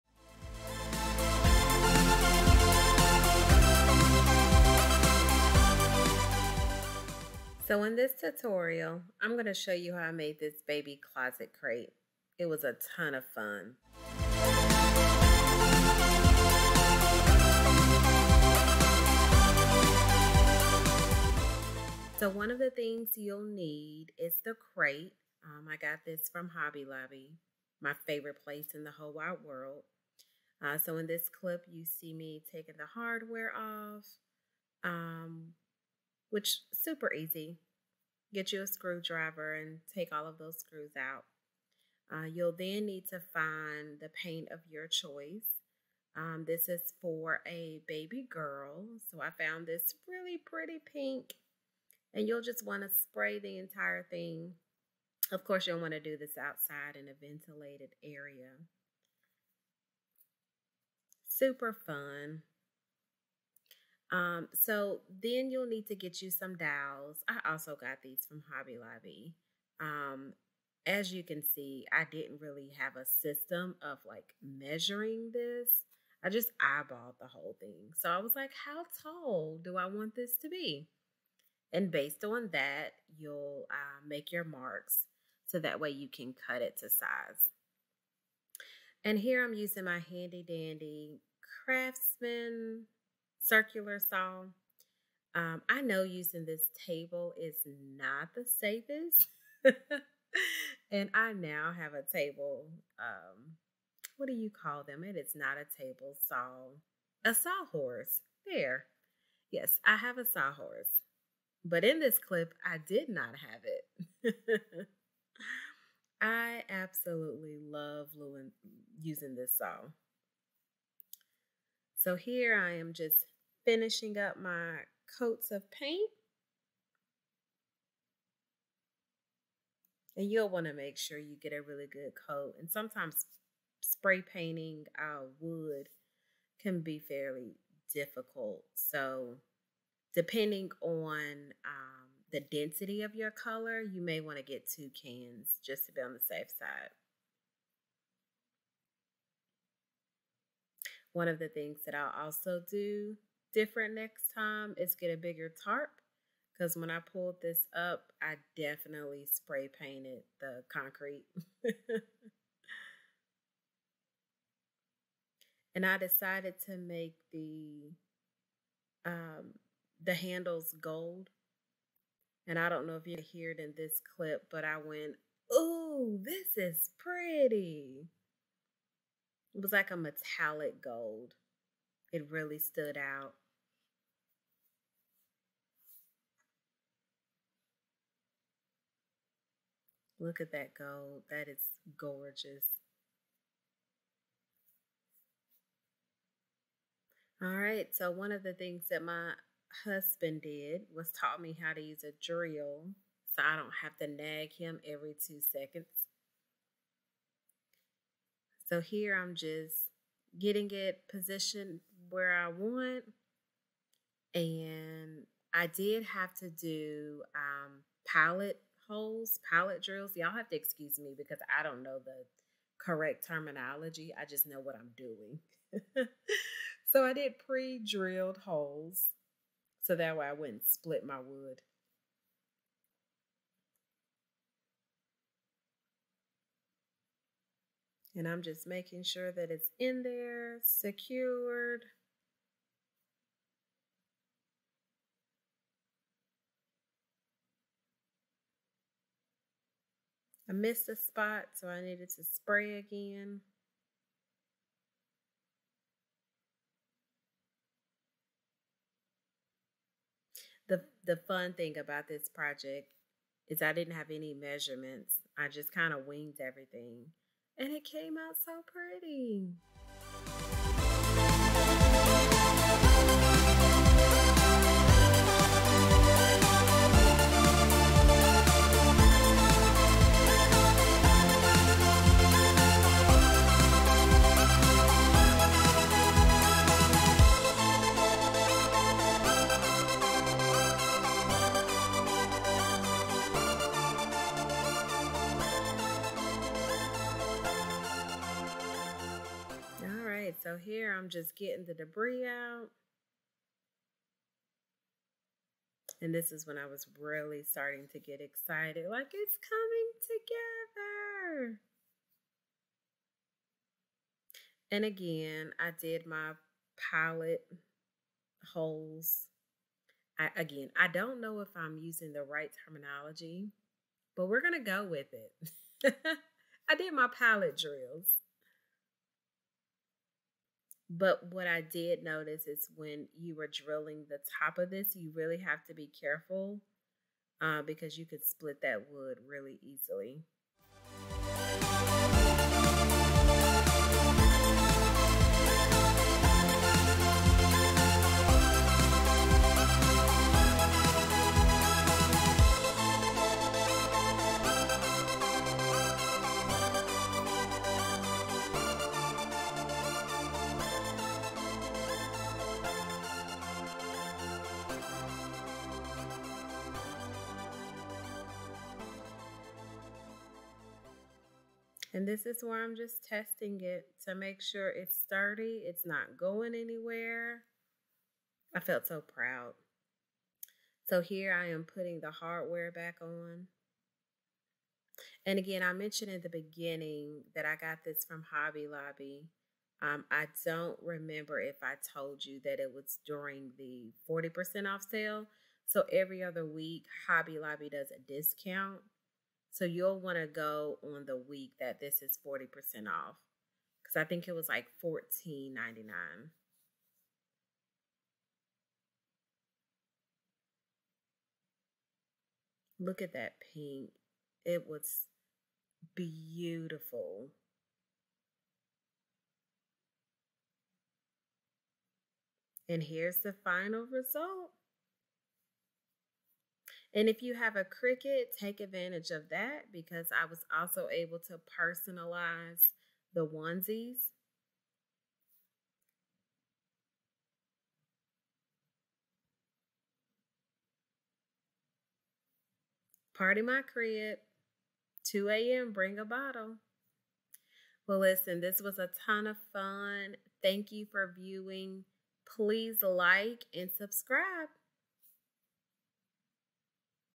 Baby Closet Crate Tutorial
Oh my goodness, I had so much fun making this Baby Closet Crate. I also bought some cotton filler and used fabric to cover the base of the crate so it would look fuller.